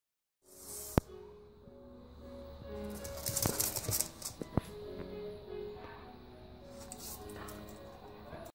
Dupp the Bearded Dragon turning into a crocodile
Dupp our male bearded dragon acting like he's being fed from a balcony like you see crocodiles being fed in zoo's awesome stuff